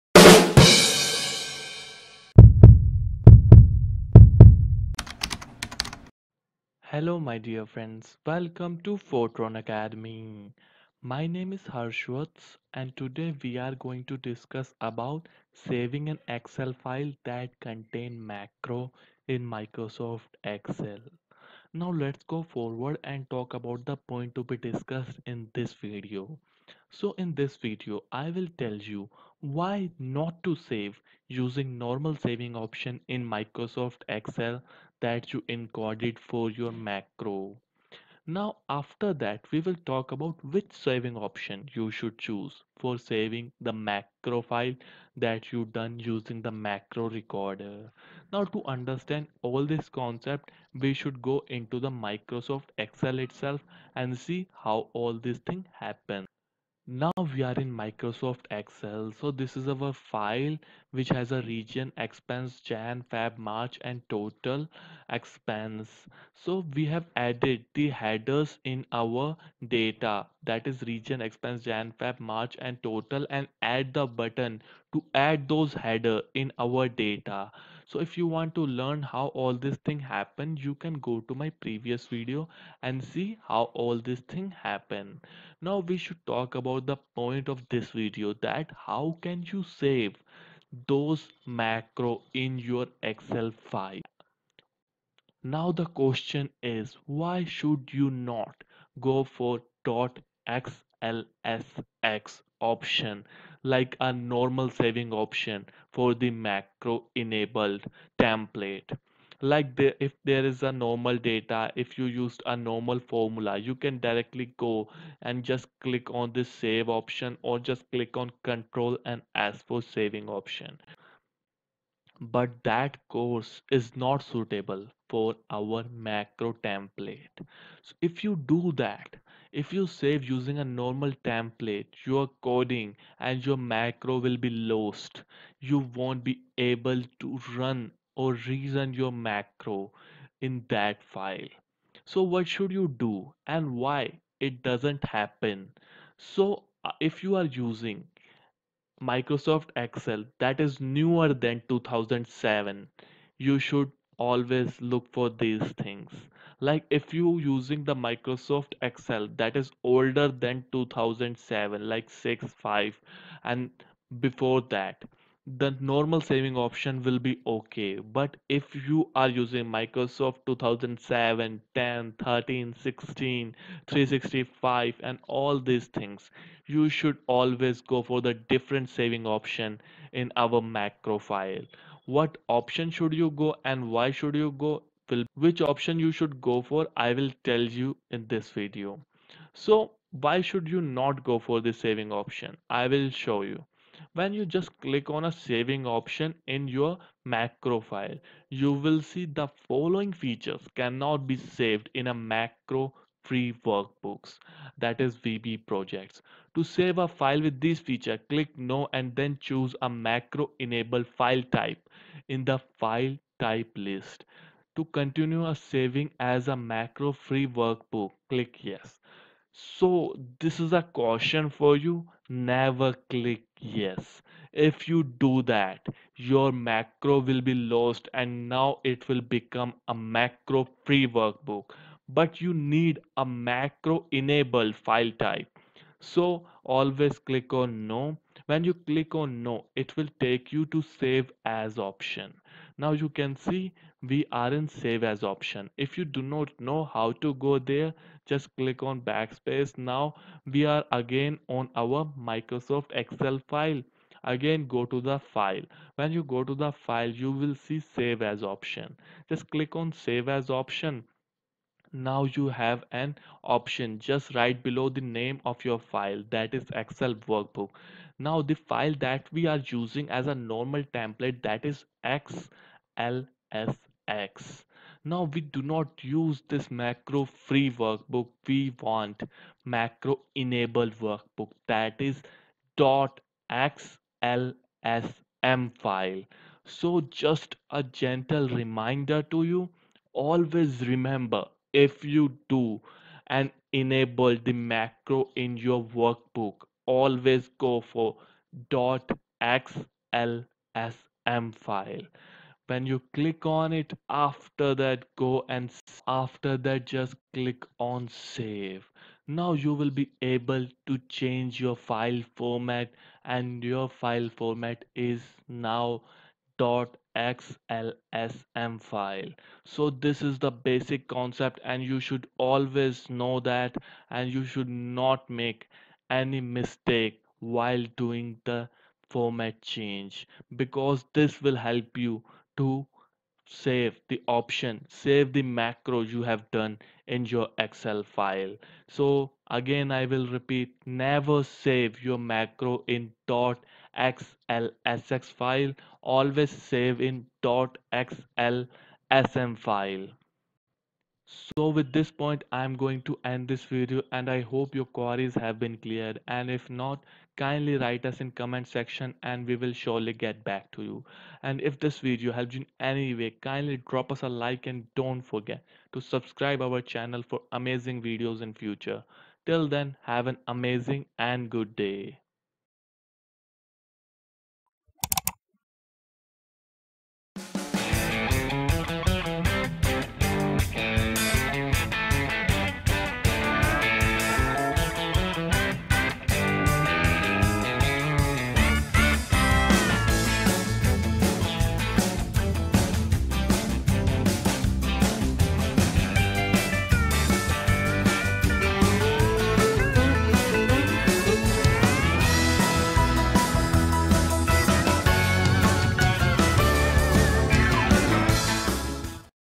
Excel VBA tutorial for beginners: How to Save a Macro Workbook
VBA stands for Visual Basic for Applications. It helps to record routine tasks. In other words, it is possible to use VBA to create Macros. Users can record Macros with a Macro recorder. The Macro recorder is simple and easier to use. It allows performing direct tasks, but not complicated tasks. Therefore, we can use VBA to expand the usual functionalities and to create advanced programs.

Points to be discussed:
• Why not save using a normal saving option?
• Which saving option you should choose for saving the Macro file?

VBA अनुप्रयोगों के लिए Visual Basic के लिए खड़ा है। यह नियमित कार्यों को रिकॉर्ड करने में मदद करता है। दूसरे शब्दों में, मैक्रोज़ बनाने के लिए वीबीए का उपयोग करना संभव है। उपयोगकर्ता मैक्रो रिकॉर्डर को मैक्रो रिकॉर्ड कर सकते हैं। मैक्रो रिकॉर्डर सरल और प्रयोग करने में आसान है। यह प्रत्यक्ष कार्यों को करने की अनुमति देता है, लेकिन जटिल कार्यों को नहीं। इसलिए, हम सामान्य कार्यक्षमता का विस्तार करने और उन्नत कार्यक्रम बनाने के लिए VBA का उपयोग कर सकते हैं।

चर्चा किए जाने वाले बिंदु:
• सामान्य बचत विकल्प का उपयोग करके बचत क्यों नहीं करते?
• मैक्रो फ़ाइल को सहेजने के लिए आपको कौन सा बचत विकल्प चुनना चाहिए?

You can access the entire course "Tutorial From Beginner to Pro ⚡ Microsoft Excel Macros & VBA"